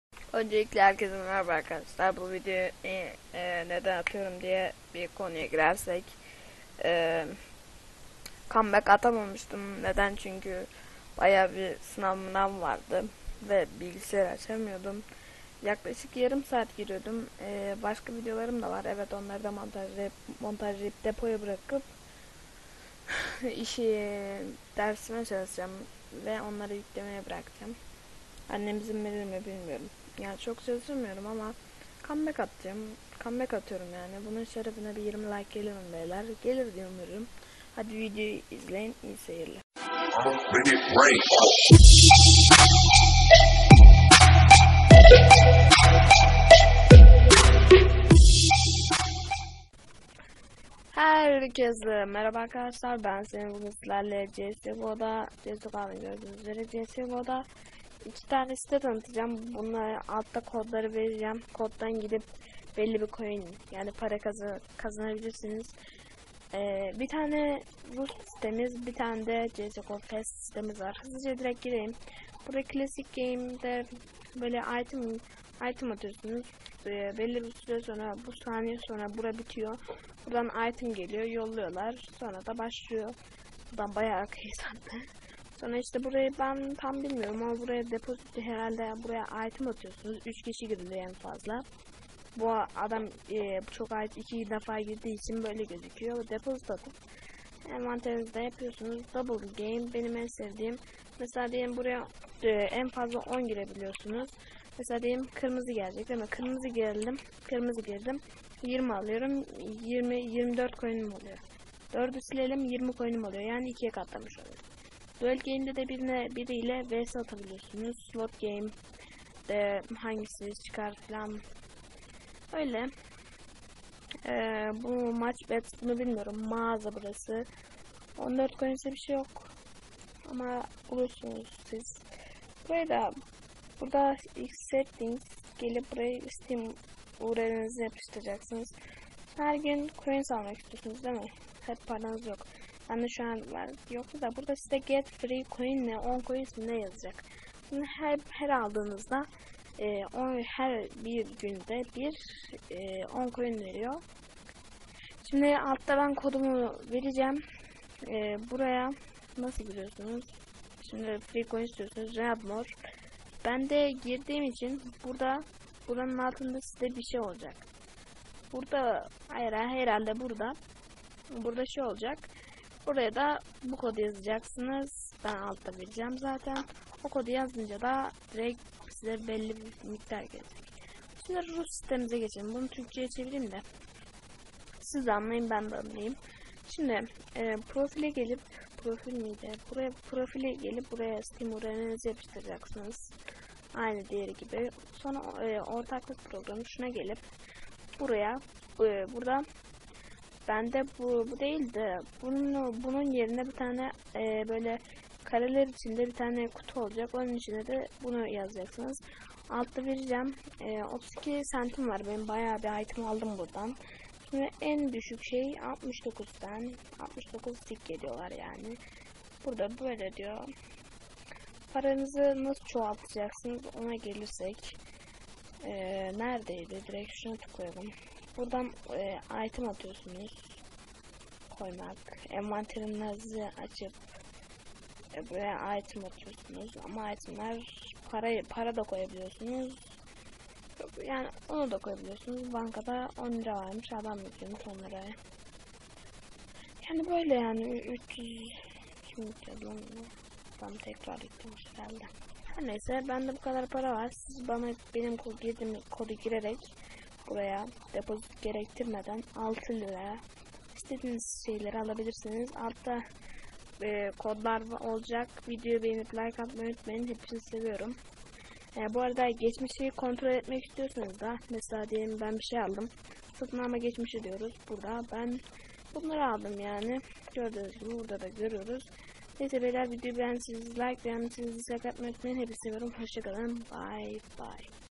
COMEBACK !! CS:GO DEPOSİT İSTEMEDEN İTEM ÇEKİLEN SİTELER
HERKEZE MERABA BEYLER BÜGÜN SİTE TANITTIM İYİ SEYİRLER
promosyon kodu : 6JYOPY
promosyon kodu : COMEBACKATTISEMIHBUBA
YAPAMADIĞINIZ ŞEY VARSA YORUM OLARAK YAZIN BEN CEVAPLARIM :)
2.nin biraz codu uzun ama nabalım :) İYİ SEYİRLER